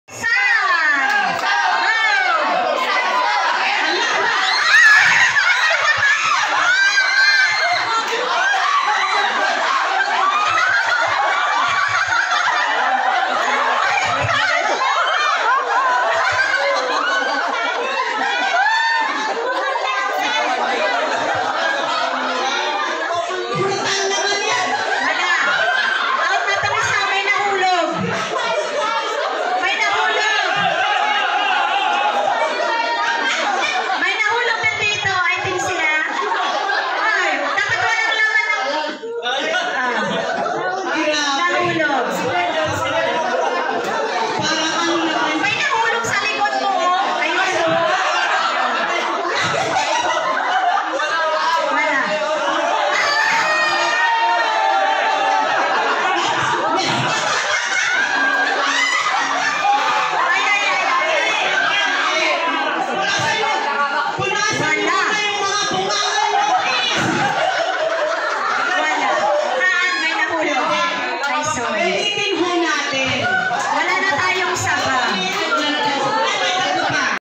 Subuan Ng Saging!
SUBUAN NG SAGING CHALLENGE
Parlor Games during the Cordillera Content Creator's Christmas Party held at Igorot Stone Kingdom, Longlong Road, Baguio City on Dec. 16, 2023.